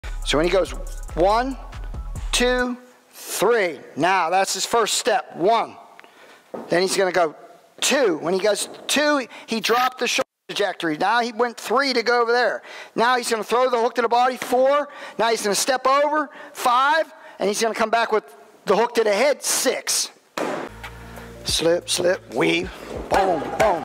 Padwork is the perfect opportunity to teach & integrate footwork & defense into your boxers training
🥊 Working the Pads: Footwork and Defense 🥊

Pad work isn’t just about throwing punches; it’s the perfect opportunity to teach and integrate footwork and defense into your boxers training. When you work the pads with your boxers, focus on them moving their feet, slipping punches, and staying defensively sound. This combination builds a complete fighter, ready to handle anything in the ring. 🦶💨🛡️

Keep those feet moving, stay sharp, and always be ready for the next challenge! 💪